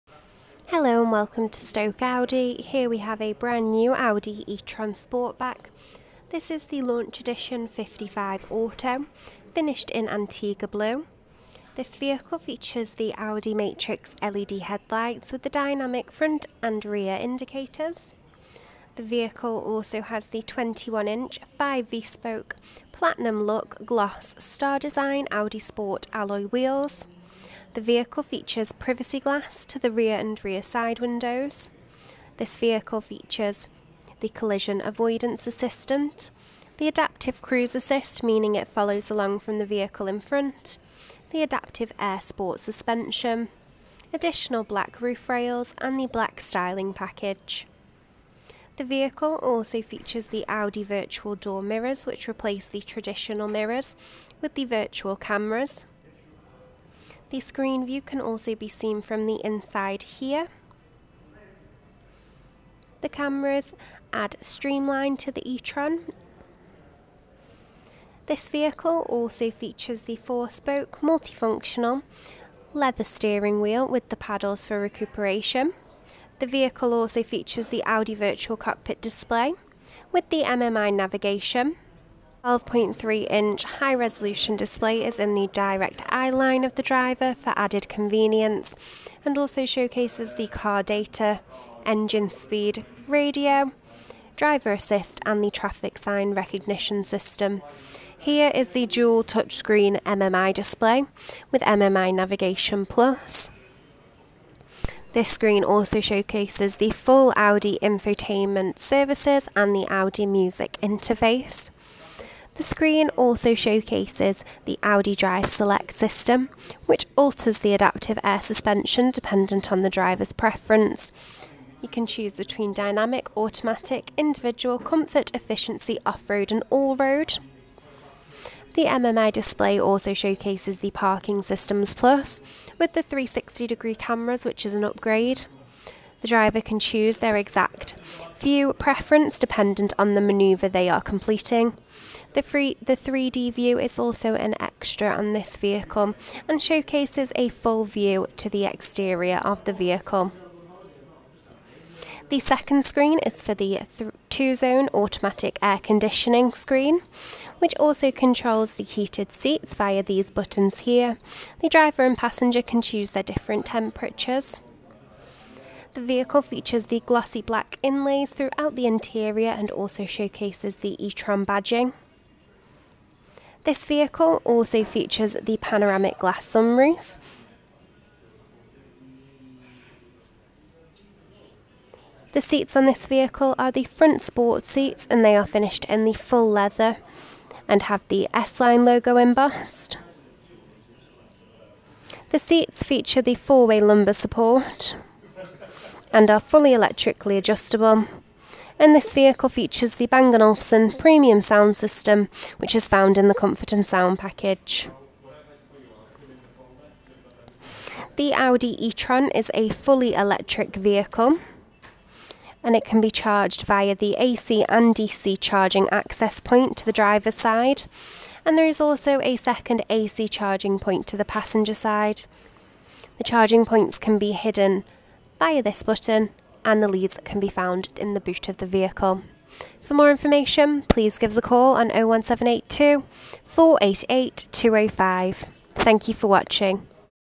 Brand New Audi E-Tron Sportback Launch Edition | Stoke Audi
Check out this Brand New Audi E-Tron 55 Launch Edition Sportback Auto Quattro, currently for sale at Stoke Audi.

Brand New Audi
Antigua Blue